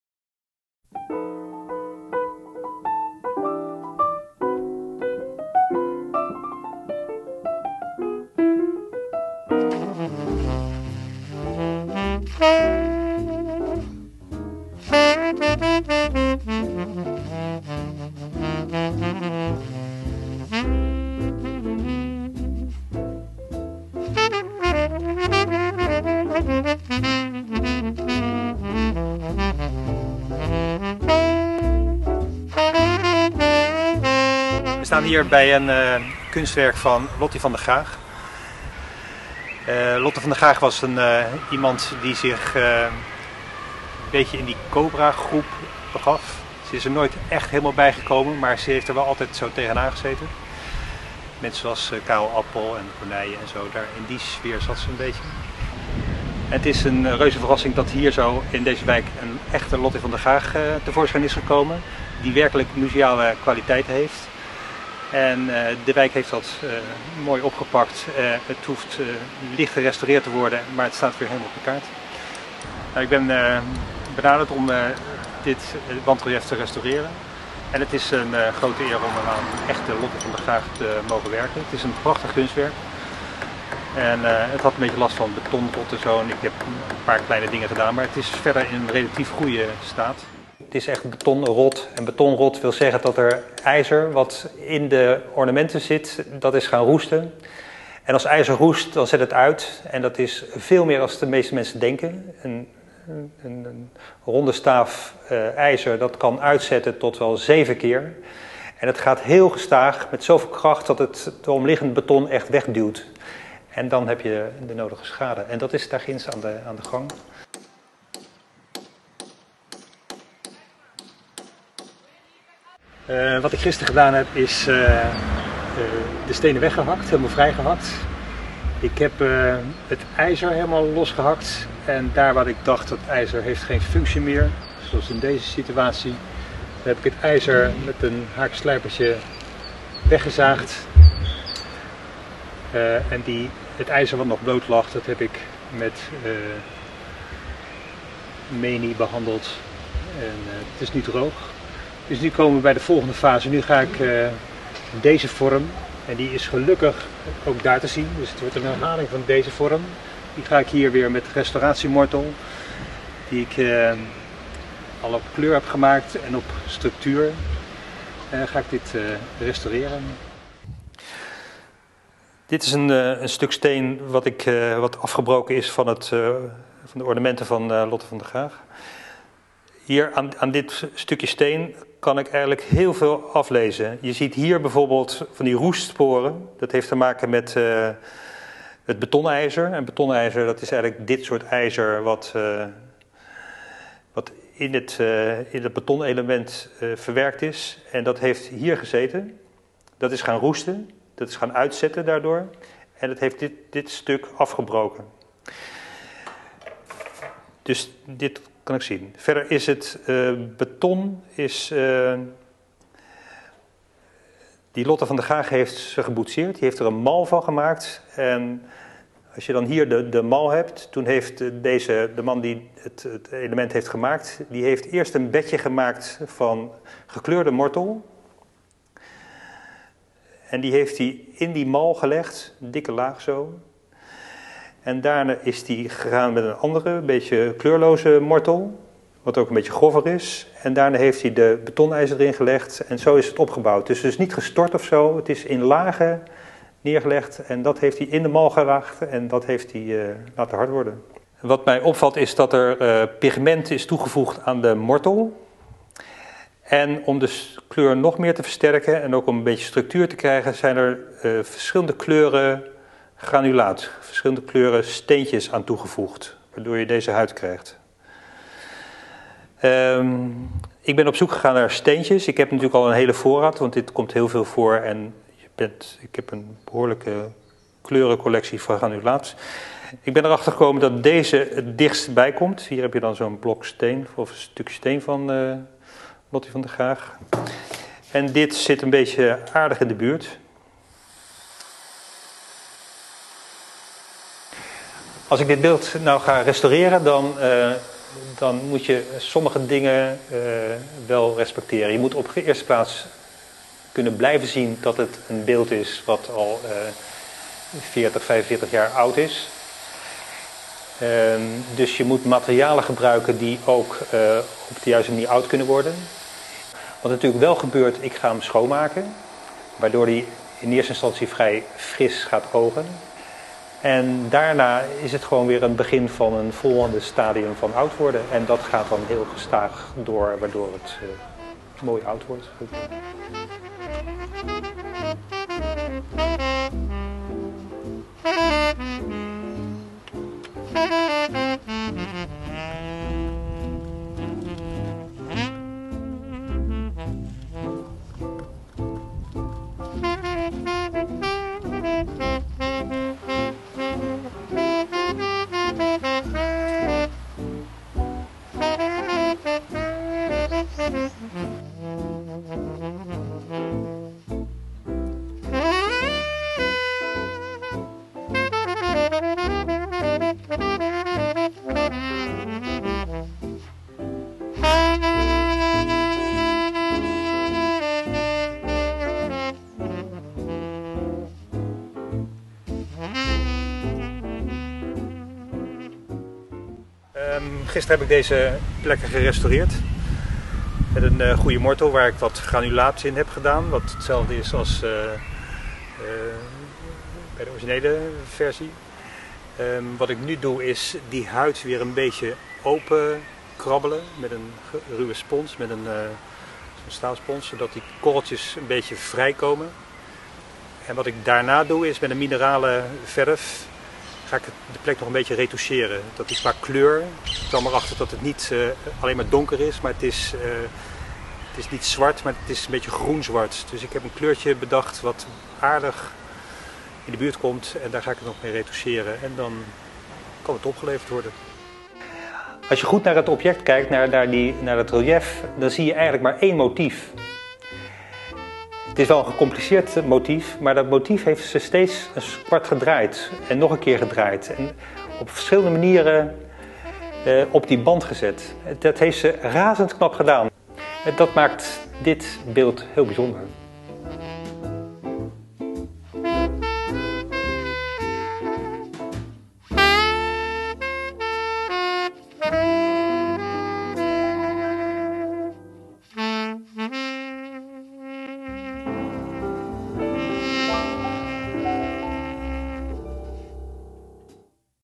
lotti, De restauratie
Videofilm over de restauratie van een wand reliëf van Lotti van der Gaag in de Zuidlarenstraat, Den Haag Zuid-West. Restaurateur Paul van Laere. Camera en montage: Henk Augustijn, art-on-video. Meer info: Theater Dakota.